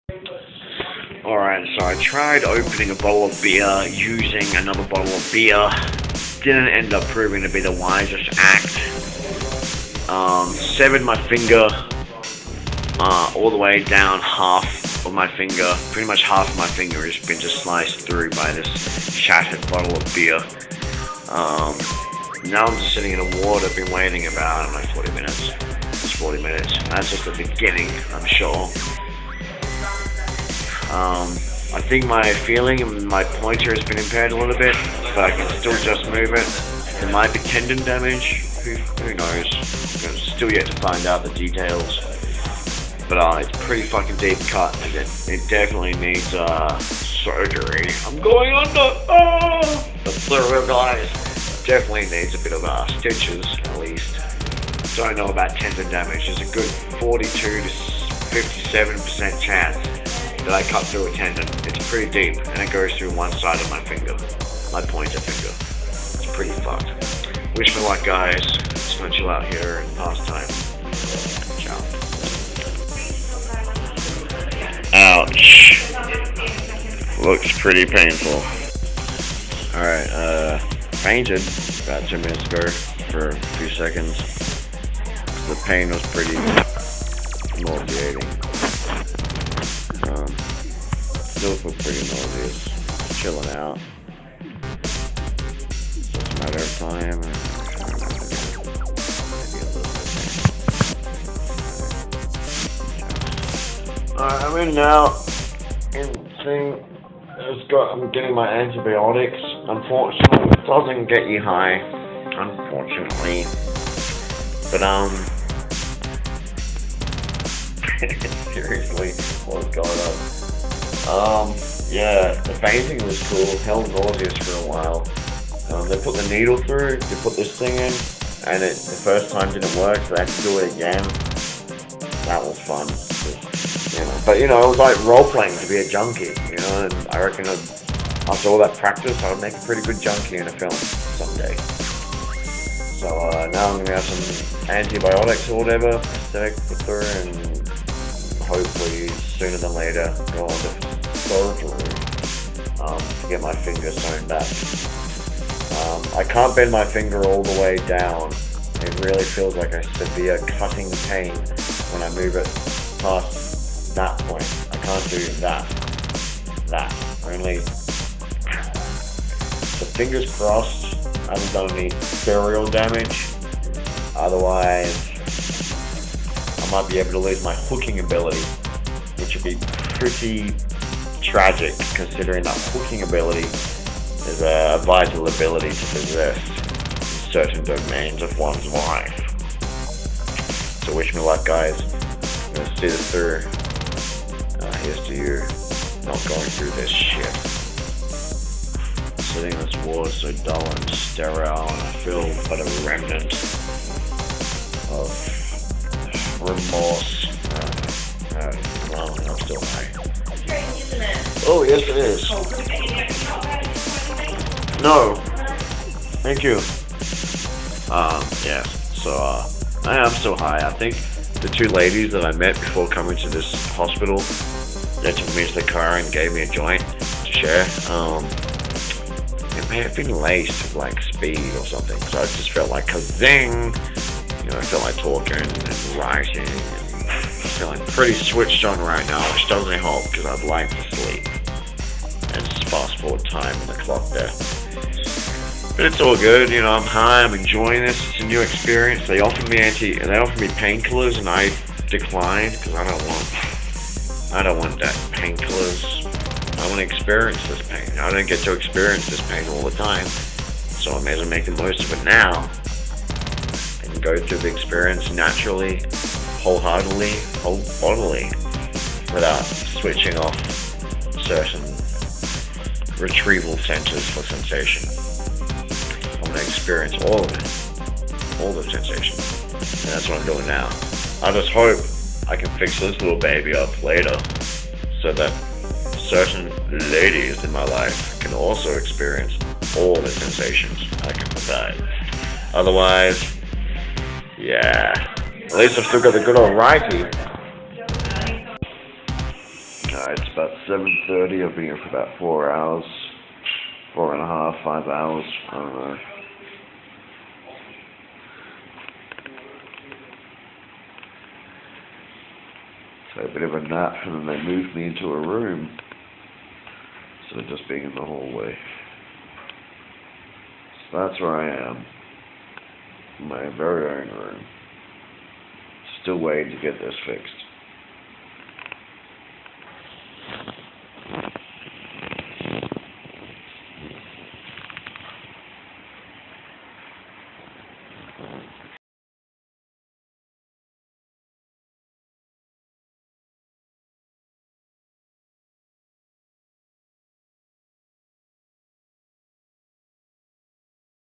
Using a Glass Bottle to Open Another Glass Bottle.. NOT a Good Idea! @_@
Remember - Metal on Glass.. does NOT work! @_@